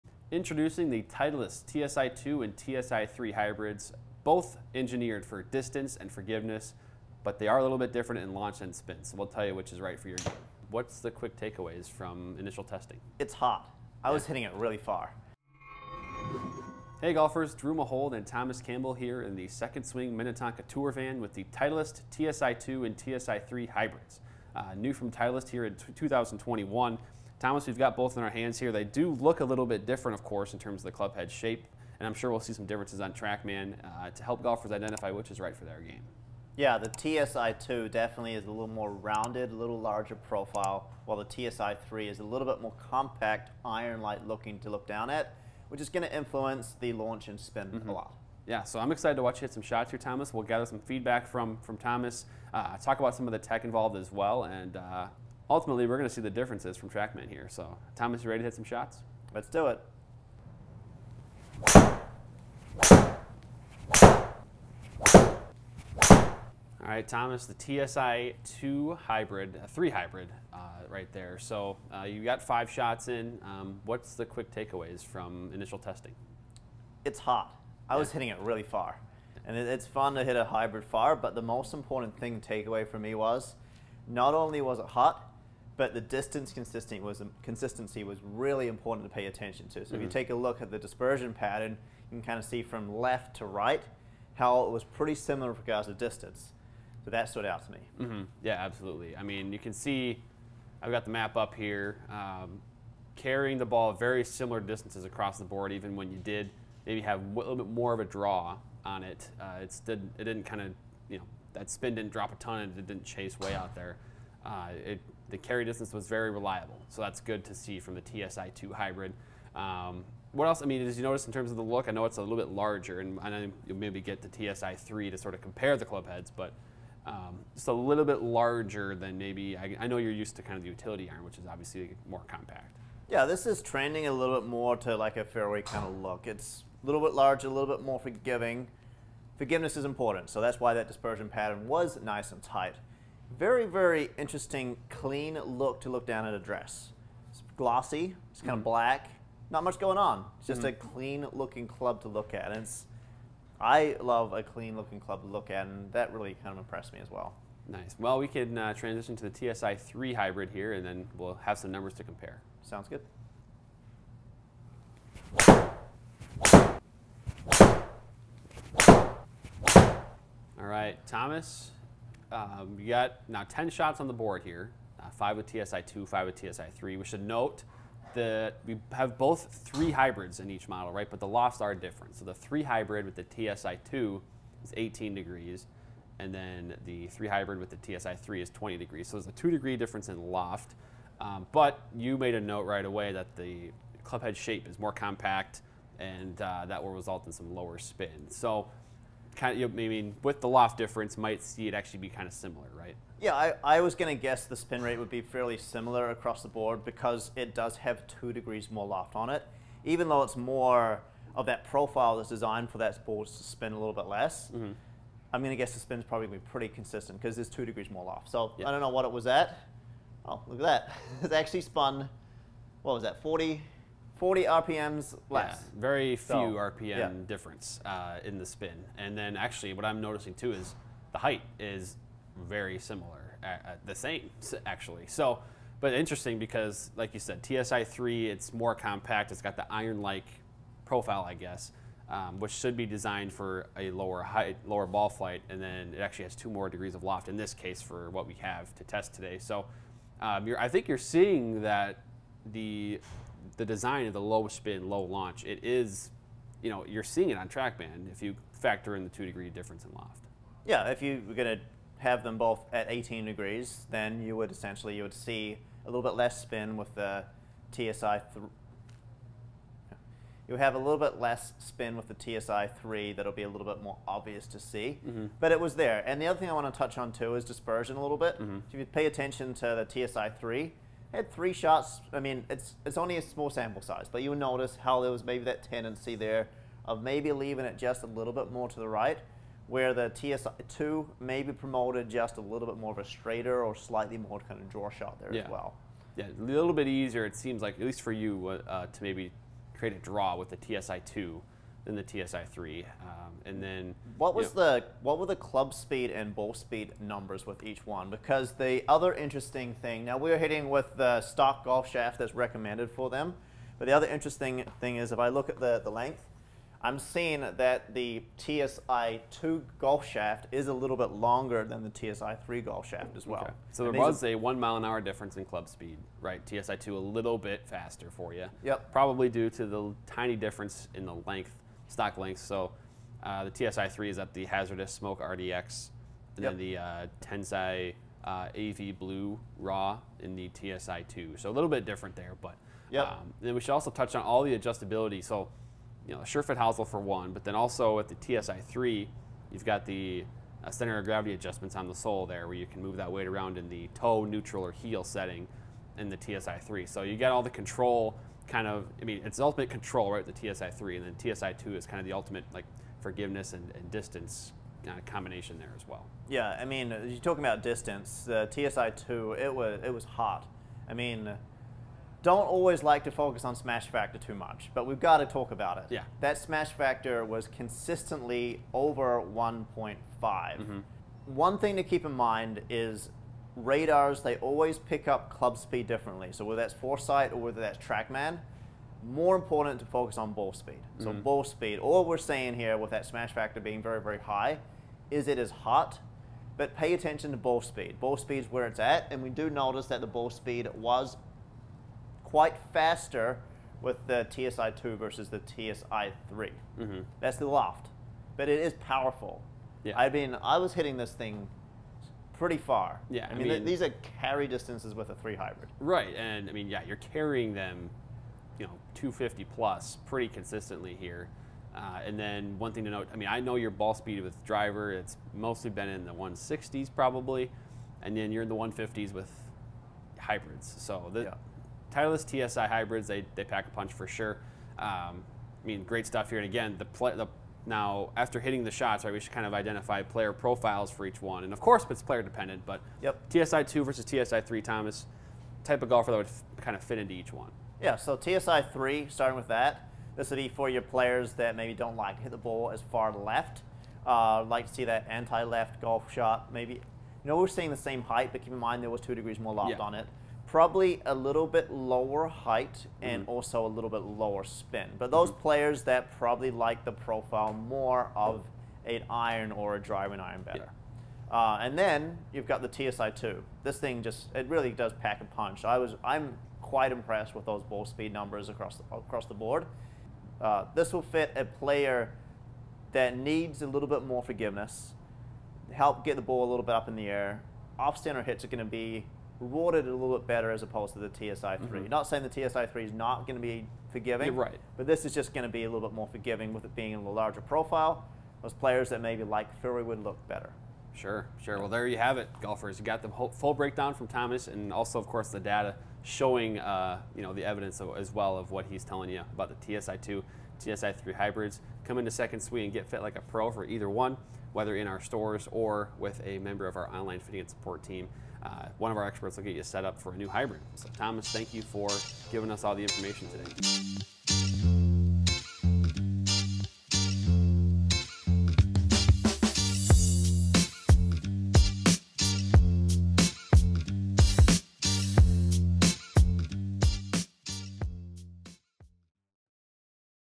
Titleist TSi2 and TSi3 Hybrids Review and Comparison | Trackman Test
The new Titleist TSi golf clubs are all designed for explosive ball speeds as Titleist continues to push the envelope on both distance and forgiveness. The Titleist TSi2 and TSi3 drivers and fairway woods have been highly successful so far, and Titleist is adding to them with the Titleist TSi2 and TSi3 hybrids.

Each model is designed for a different type of player. In this video, 2nd Swing's Drew Mahowald and Thomas Campbell identify which player types will fit into the Titleist TSi2 and Titleist TSi3 hybrids with the help of Trackman.